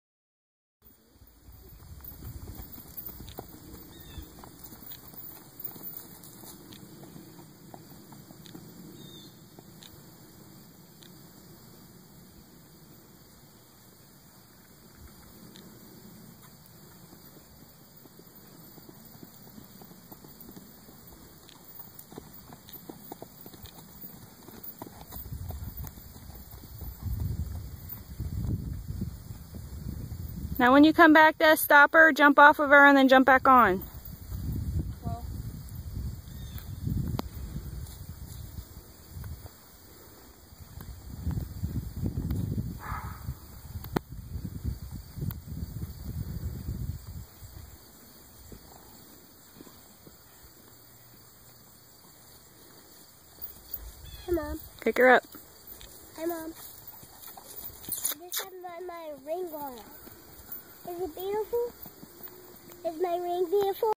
Registered TWH. 16-2 hh sorrel mare
Registered 5 year old TWH mare. She is about 16-2 hh. This is my 13 year old riding her. She was taken out of the field and this is how she did. She hasn't been ridden in over a year.